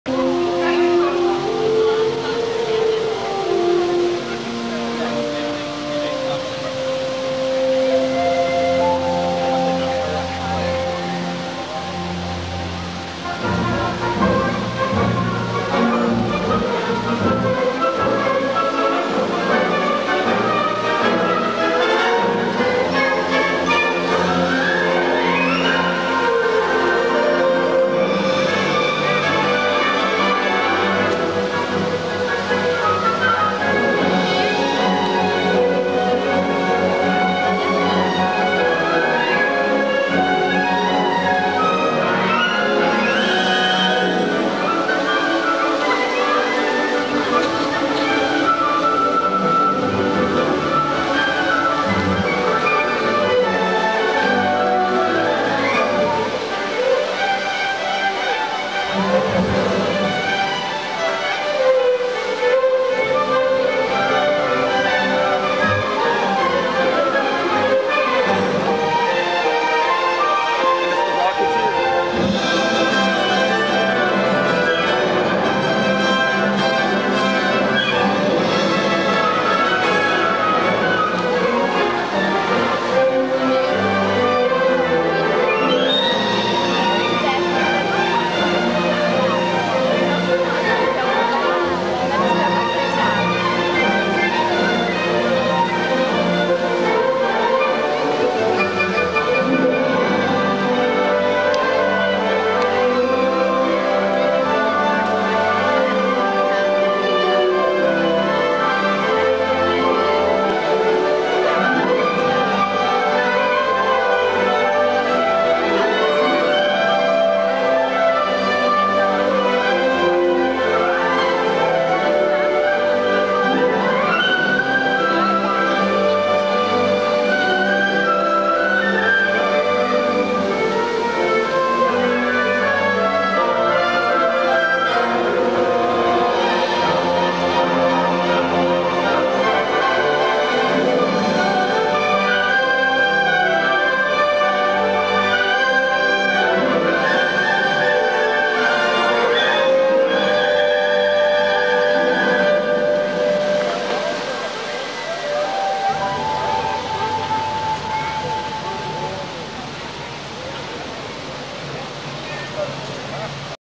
Fountain show at Epcot, 1998
Every quarter hour the Fountain of Nations in Innoventions Plaza performs an automatic water jet show.  This video was recorded in July, 1998- before the big "Epcot" wand was installed (and later removed).